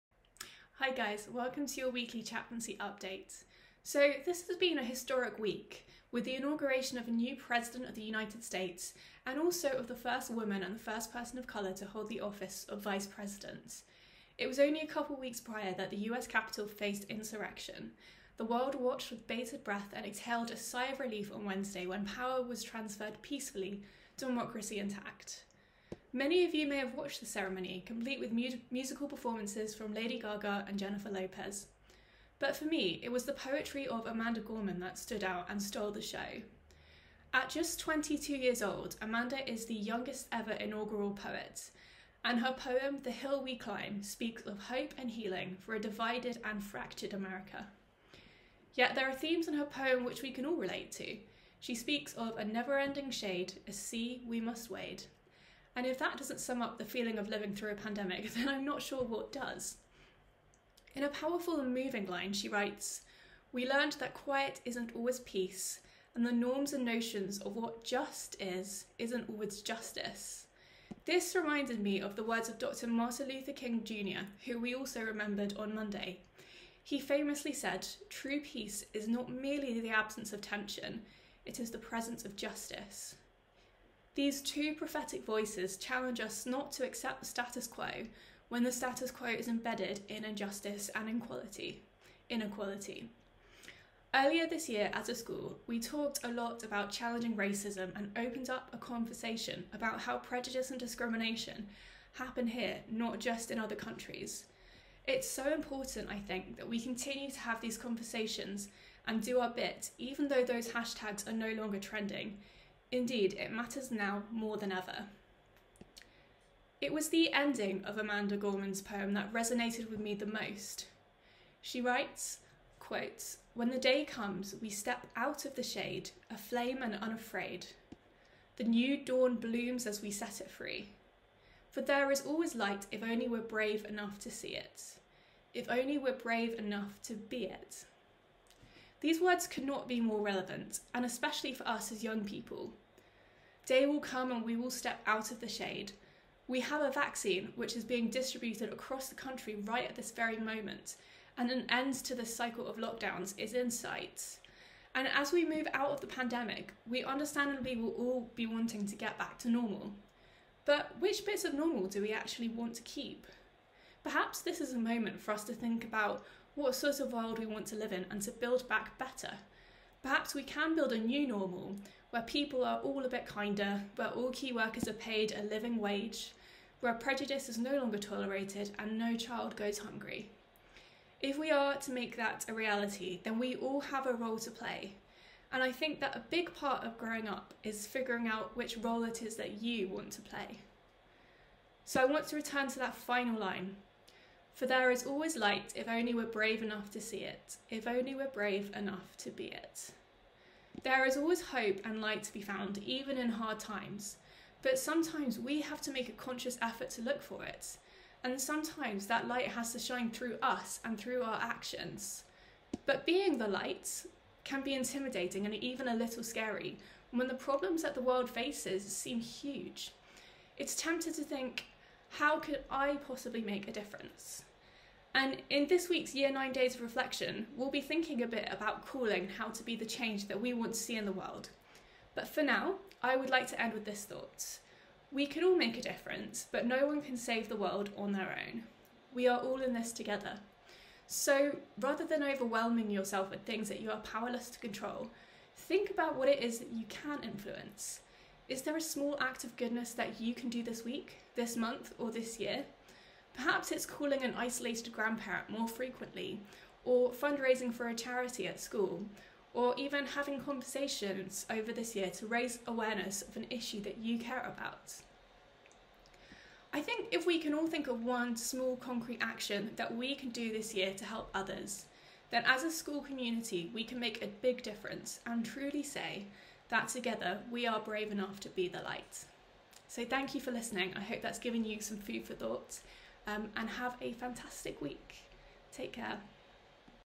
The Hill We Climb
In this week's chaplaincy update, Mary takes a look at "The Hill We Climb", the inaugural poem written by 22 year old Amanda Gorman. This poem is full of themes of hope and healing, and this video looks about how we can respond to Gorman's challenge that we be brave enough to be the light.

Don't forget if you want to stay in touch with the Chaplaincy Team you can do so in the following ways: